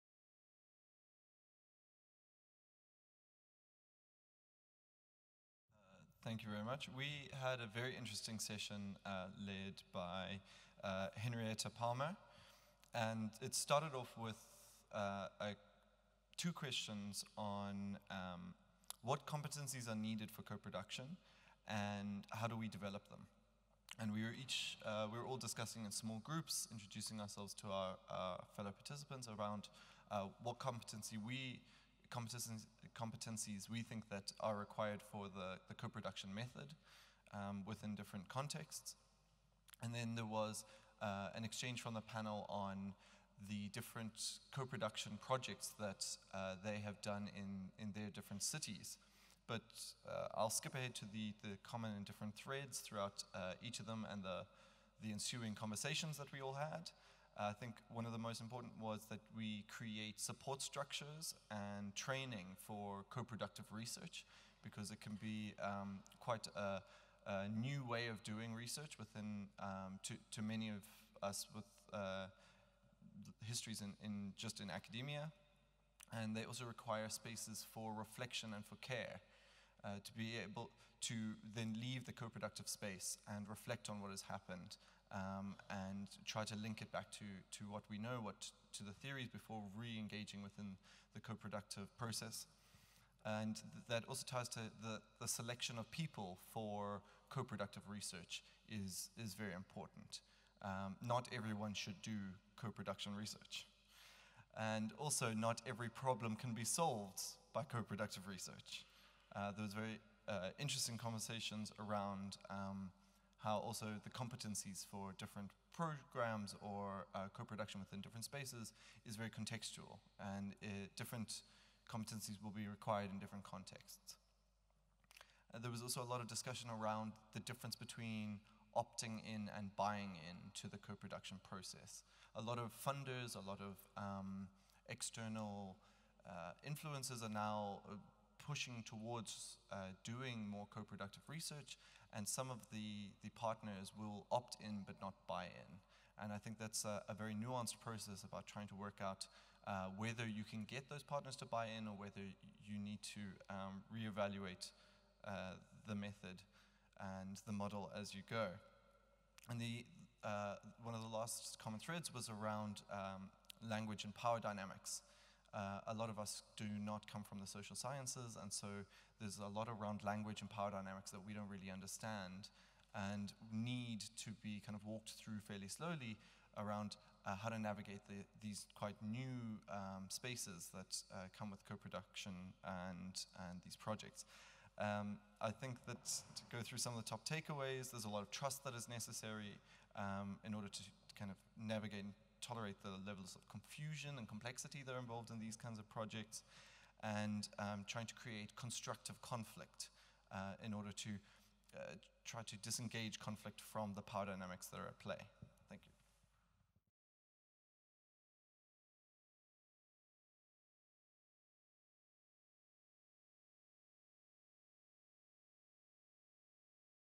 4: Building capacity for co-production: training the next generation of scholars and practitioners
Reporting back from the Realising Just Cities 2019 conference session on Building capacity for co-production: training the next generation of scholars and practitioners, Sheffield, 15 October 2019.

The session itself was hosted by Henrietta Palmer with input from Zarina Patel, Eva Maria Jernsand, Maddy Hubbard (Sheffield-Manchester), Katie Finney (Sheffield-Manchester) and Jeni Vine (Sheffield). (Rapporteur: Sean Cooke)

Video produced for the Urban Institute, University of Sheffield and Mistra Urban Futures.